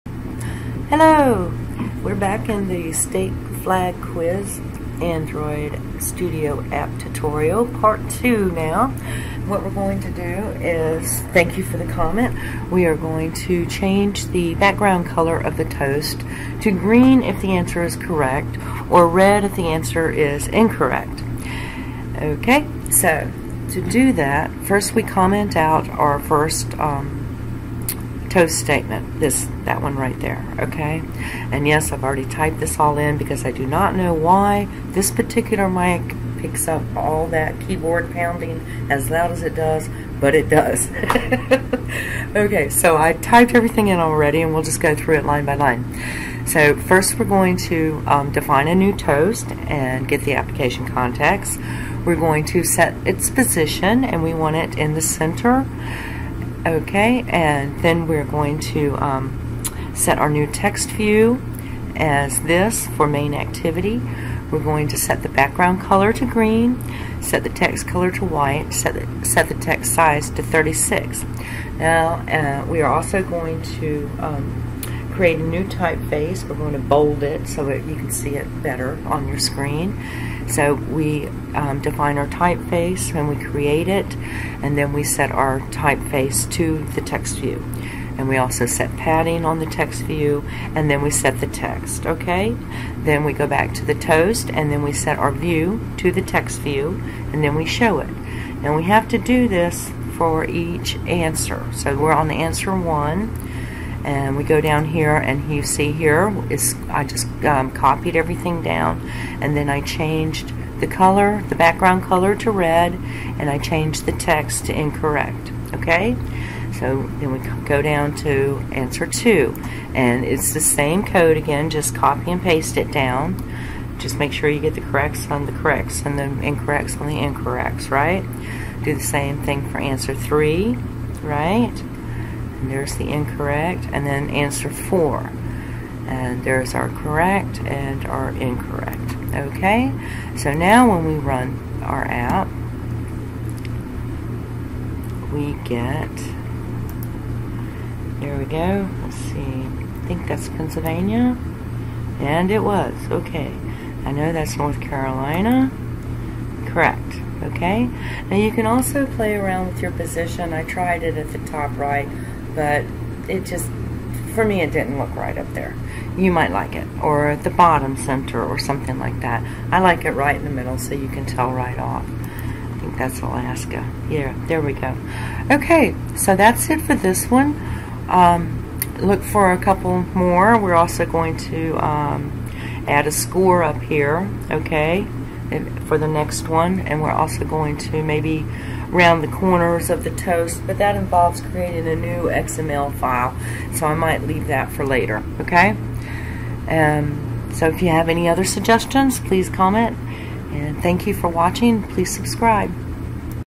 State Flag Quiz Android Studio App Tutorial Part 2
Shows how to customize the toast message, adding a green background for correct answers and a red background for incorrect answers.